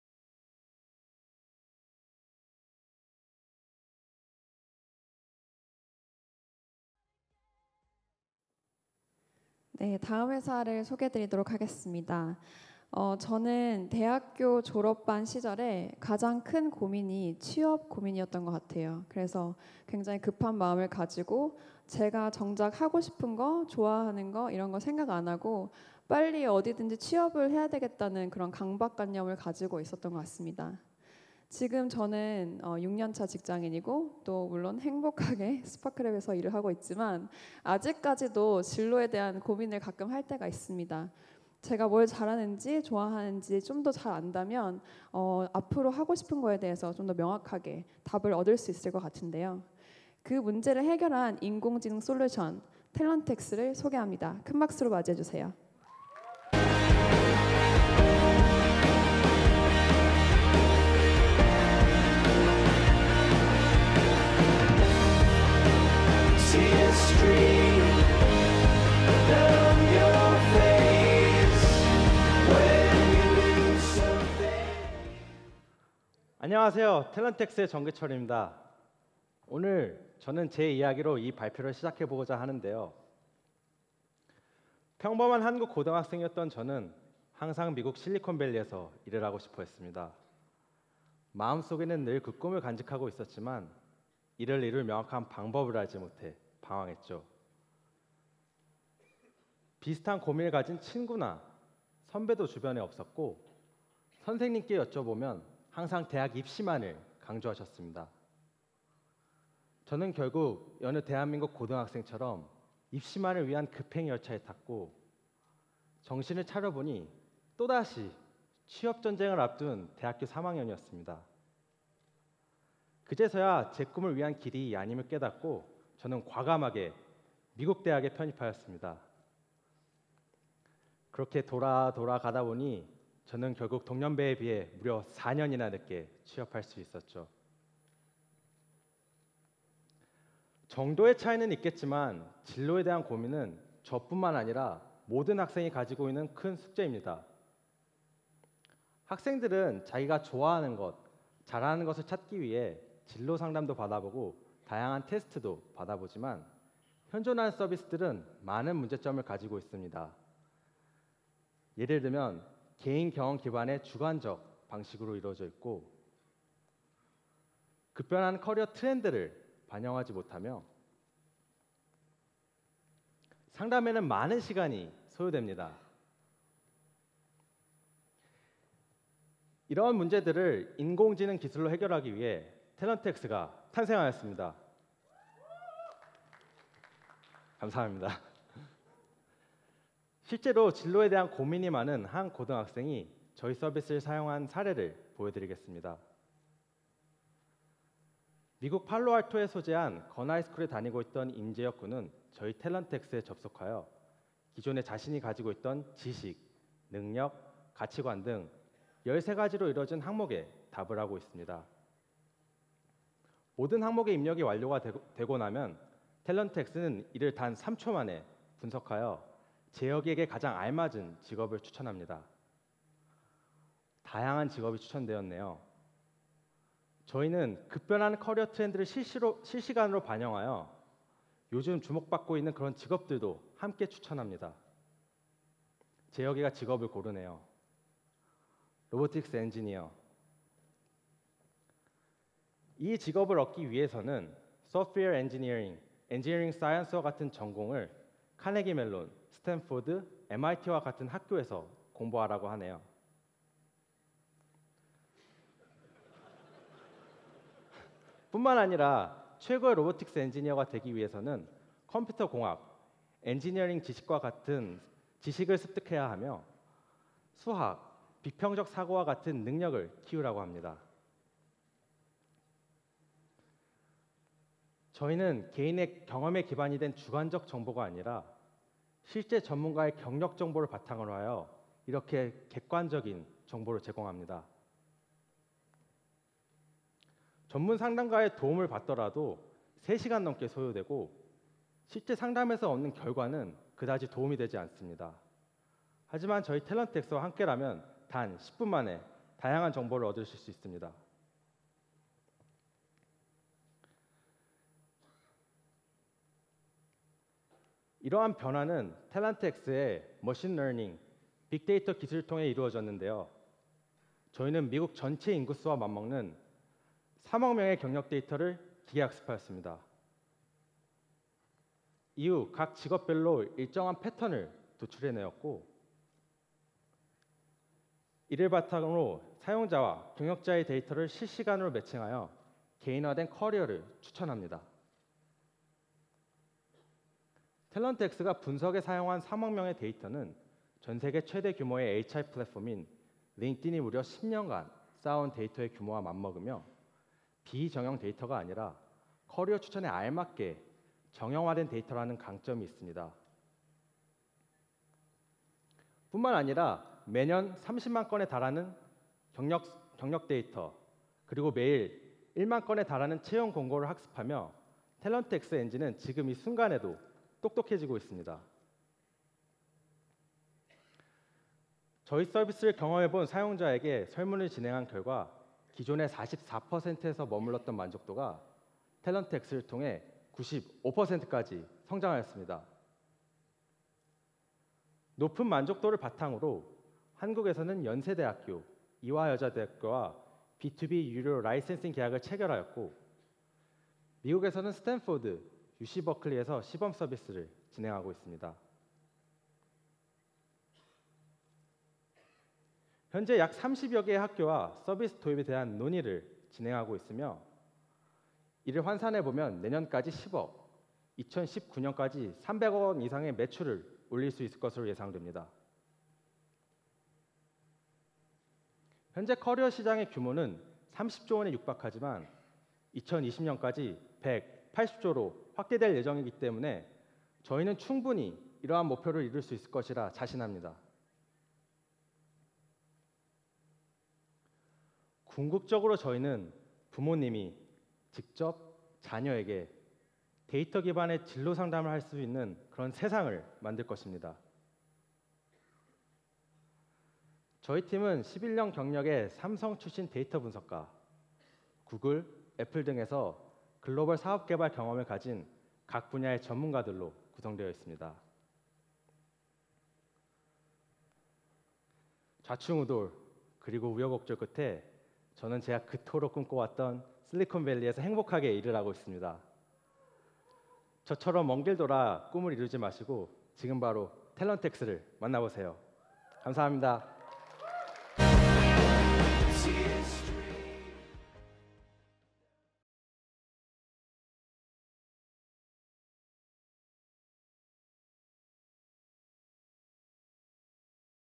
SparkLabs Demoday 8_DreamSquare
DreamSquare is an AI technology based company in Silicon Valley. DreamSquare has launched the robot career advisor platform called Talent X to help students find their right career path. Within the platform, students can navigate more opportunities that fit with their personal traits and strength.